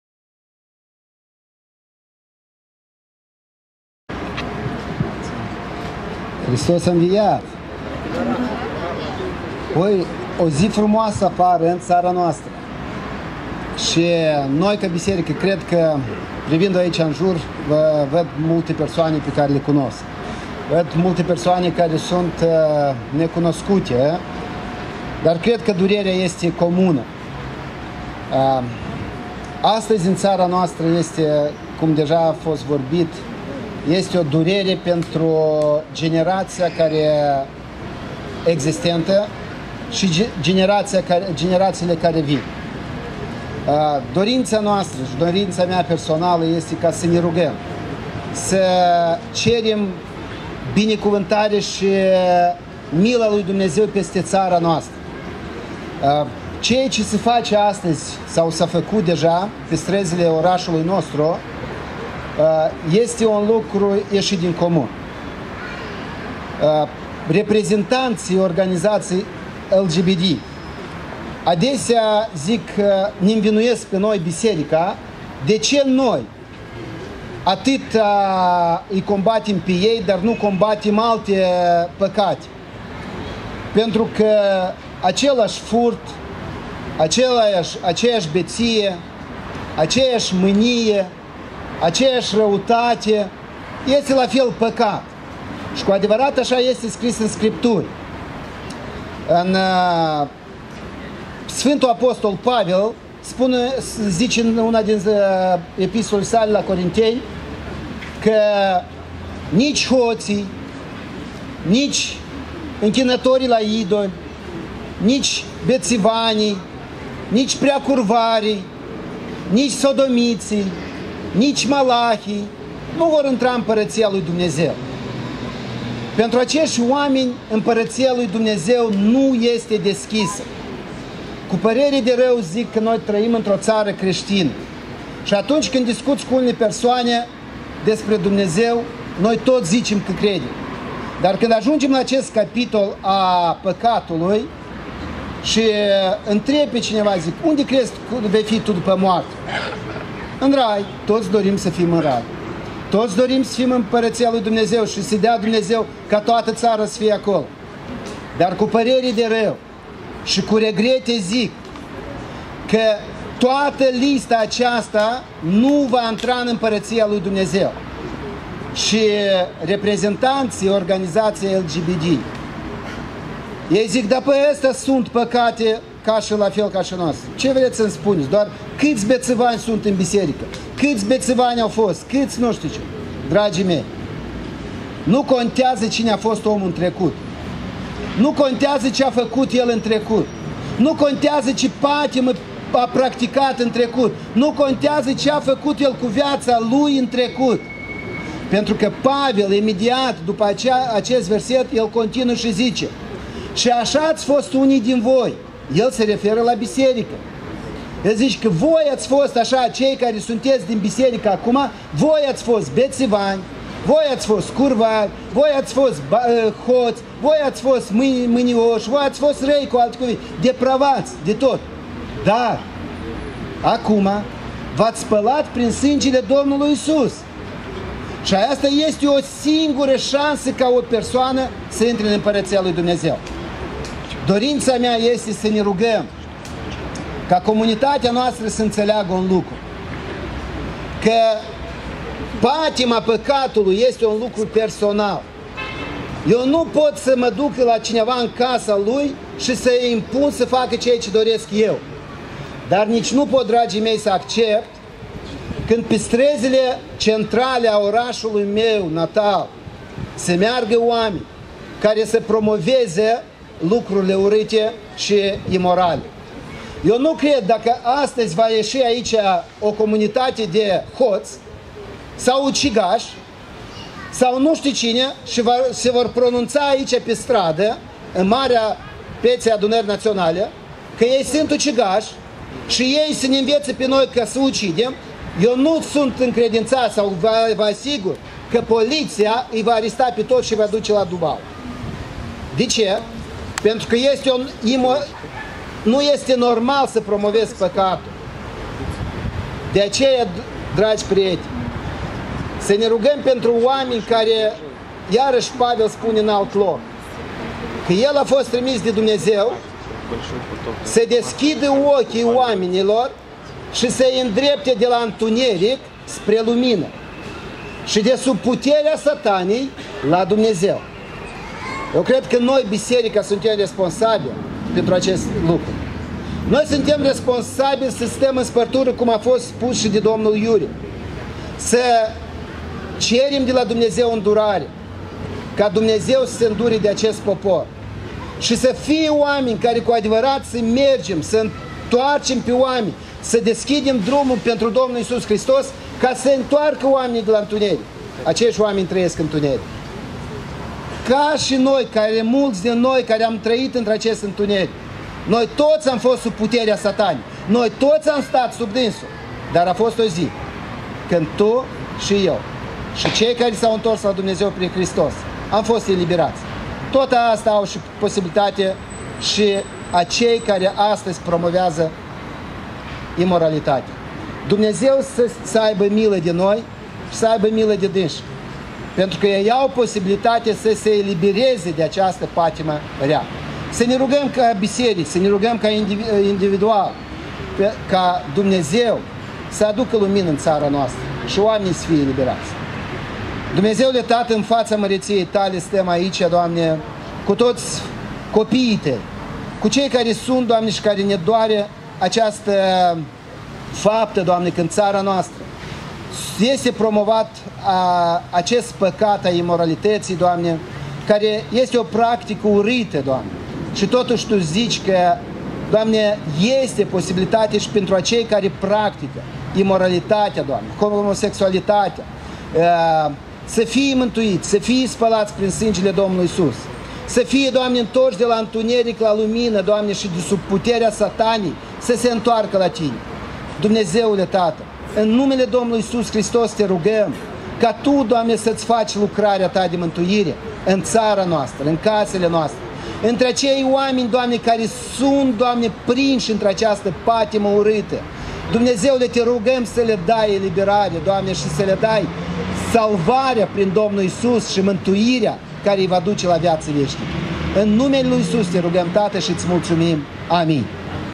Valeriu Moisei, la întrunirea de rugăciune pentru cei afectați de homosexualitate
Valeriu Moisei, profesor la Institutul de Studii Biblice Inductive din Eurasia, s-a rugat pentru pocăința persoanelor LGBT la întrunirea de rugăciune, organizată de Asociația Pentru Familie în comun cu Biserica Buna Vestire Chișinău, în data de 17 mai 2015.